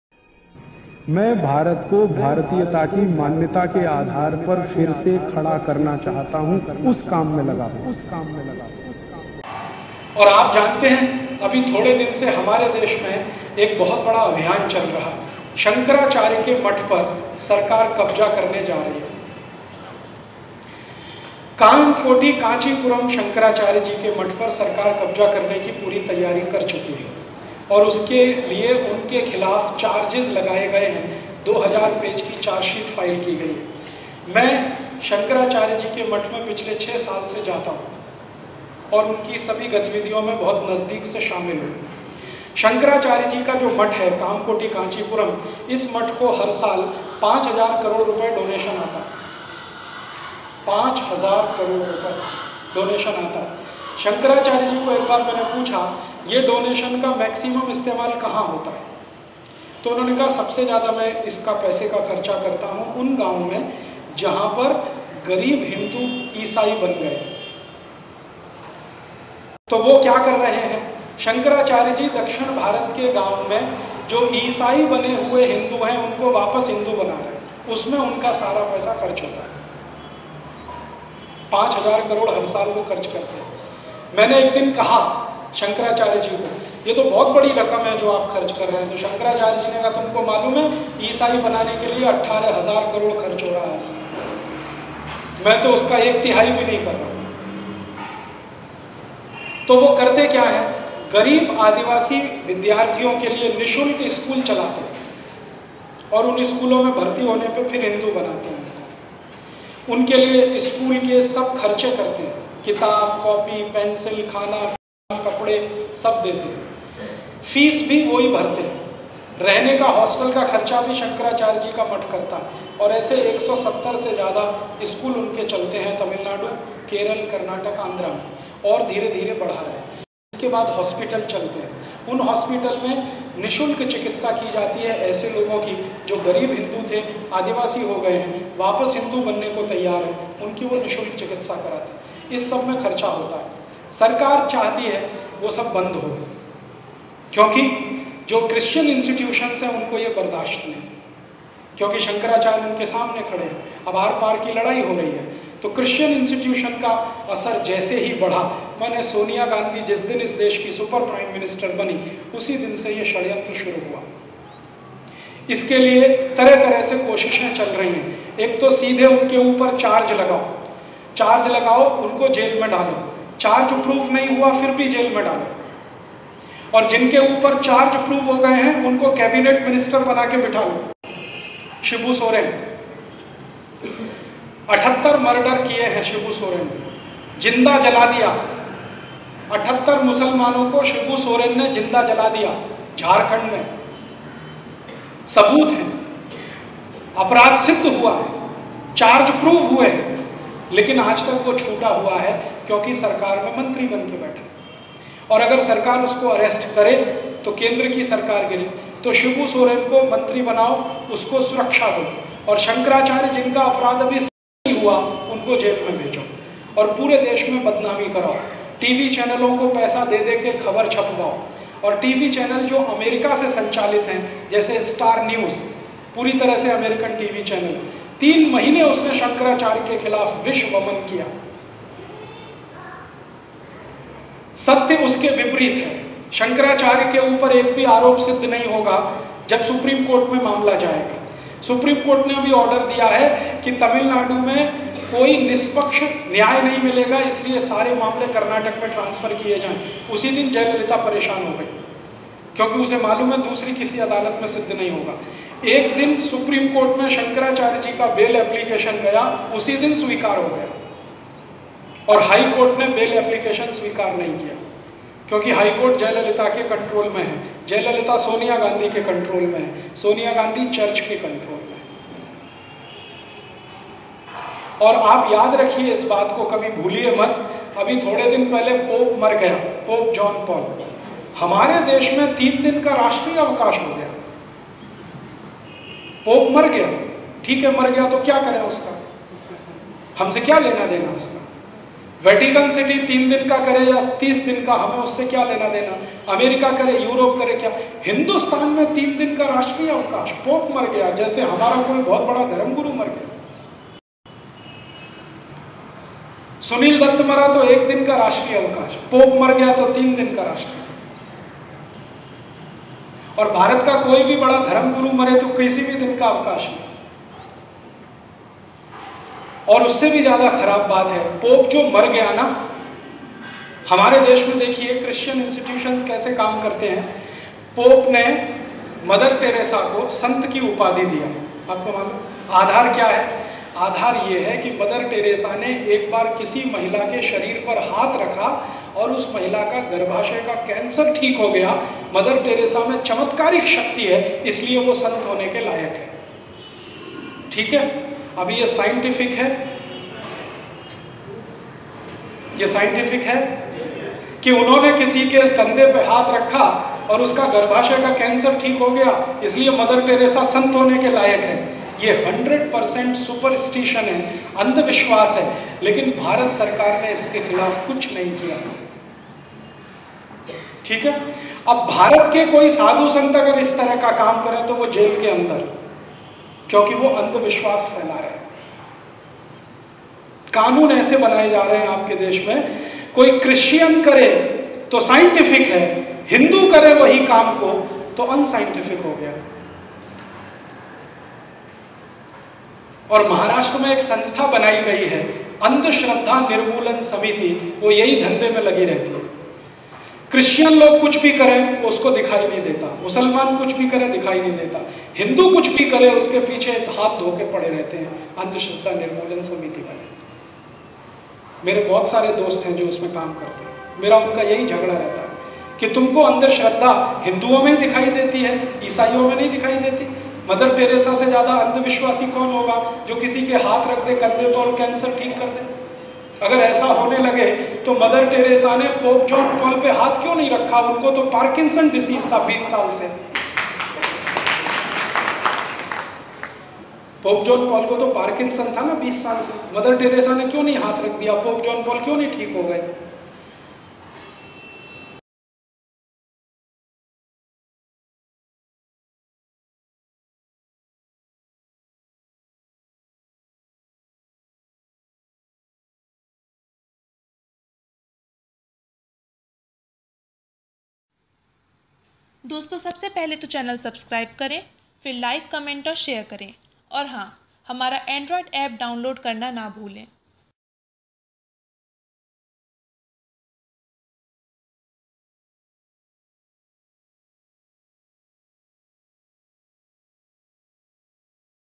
चर्च के इशारों पर हो रही धीरेन्द्र शास्त्री की बदनामी ? Bageshwar Dham - Rajiv Dixit
भारत में एंटी हिन्दू NGO कैसे काम करती है उसके बारे में इस विडियो में बताया गया है. वो धर्म परिवर्तन के लिए गरीब हिन्दुओं को लालच देती है और राजनेतिक दबाव बनाकर भी साधू संतों पर मुक़दमे दर्ज करवाकर उन्हें फसाती है. कांचीपुरम शंकराचार्य को भी राज्य सरकार ने ऐसे ही एक केस में फंसाया था.
trendingshorts trend trendingstatus tredingviral tredingcoloureeffectvideo